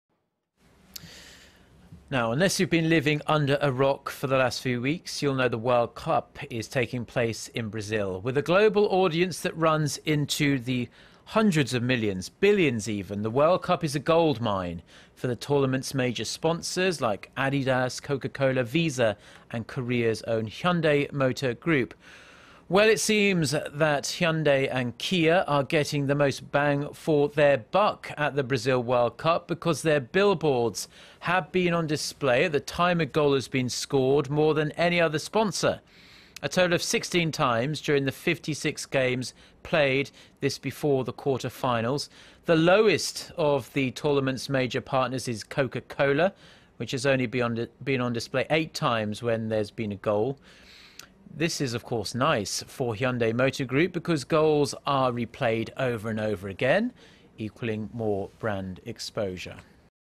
Hyundai Kia enjoy best brand exposure at Brazil World Cup
Unless you've been living under a rock for the last few weeks,... you'll know the World Cup is taking place in Brazil.